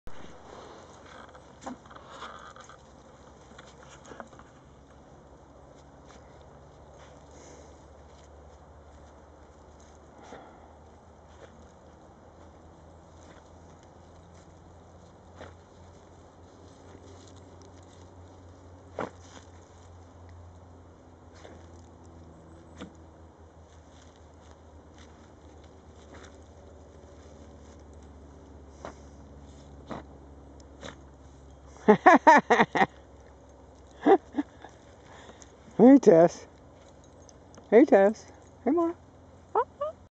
Cairn Terriers after mice in my raised bed
Three siblings digging for Mice. You can see who the really interested ones are by the wagging taoils. Brownie is checking it out because the others did.